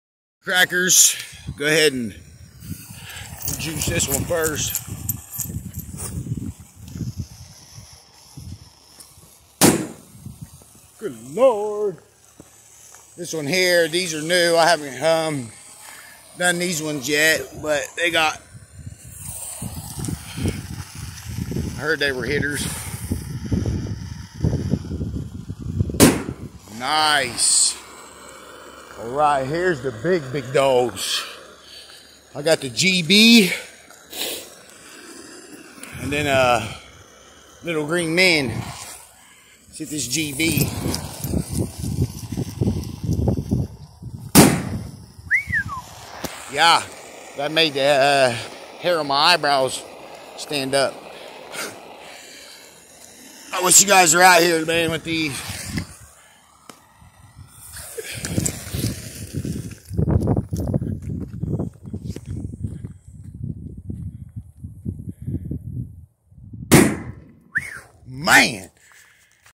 🧨Big Firecrackers🧨
just a few big firecrackers I got in my collection be safe out there pyros